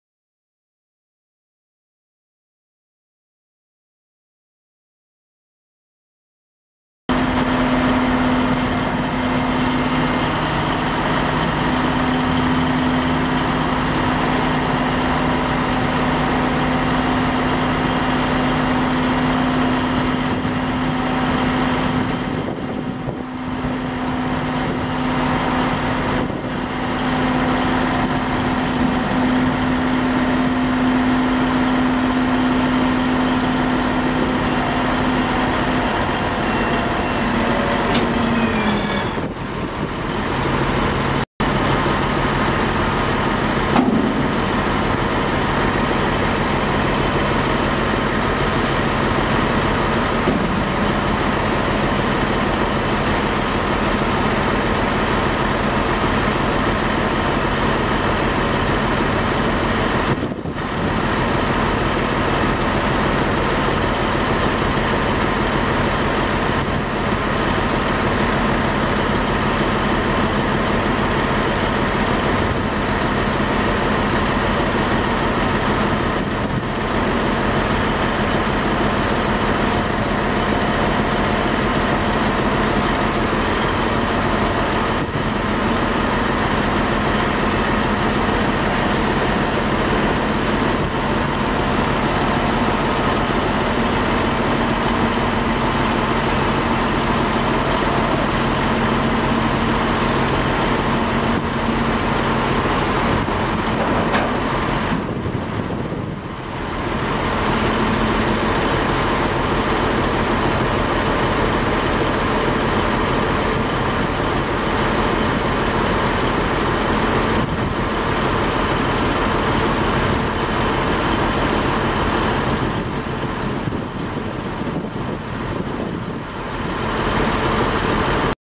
1994 Clement end dump trailer for sale | sold at auction August 14, 2014
1994 Clement end dump trailer
26&#039;L
Single ram five stage hoist
Pneumatic tailgate latch
Electric roll-over tarp
11R24.5 tires
Rust
1C9AB26B7RM110420